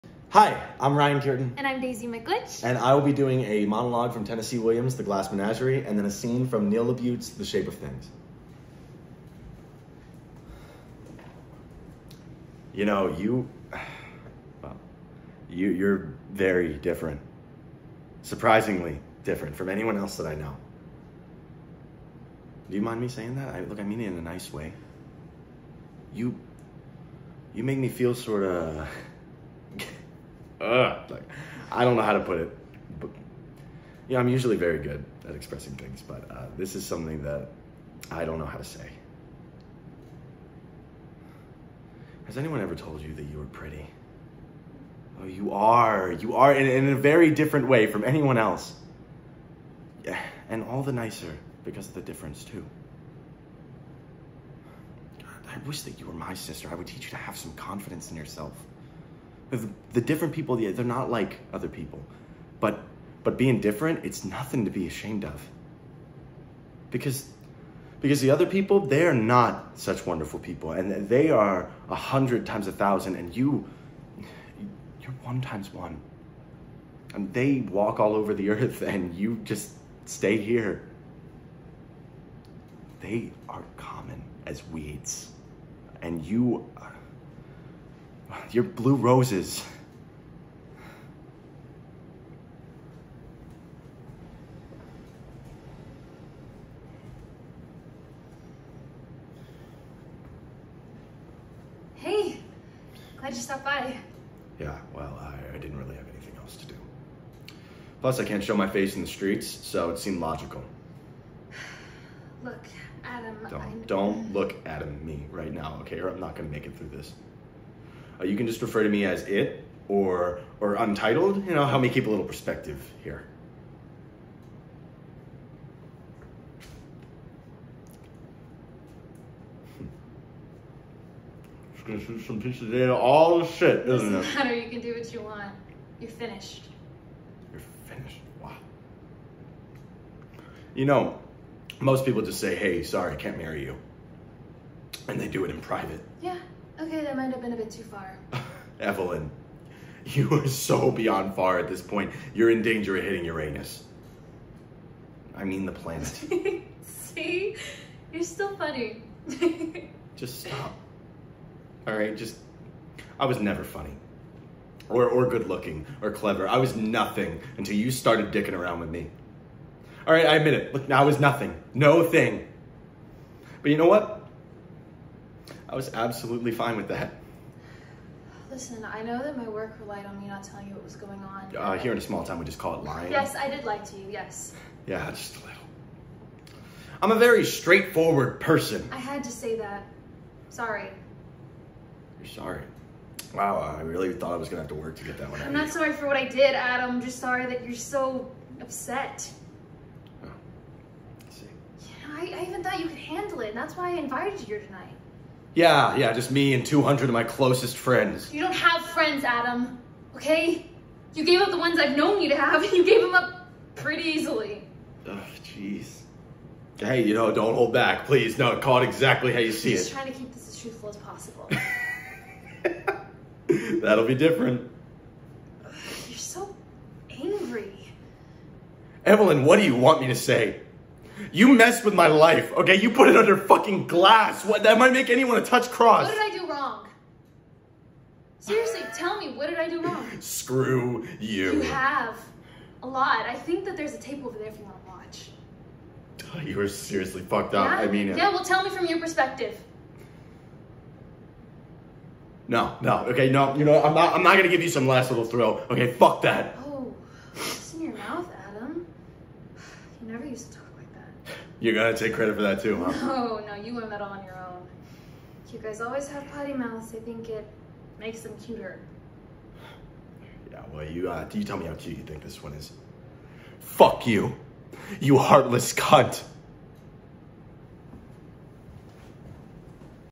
Irene Ryan Submission
A monologue from The Glass Menagerie and a scene from The Shape of Things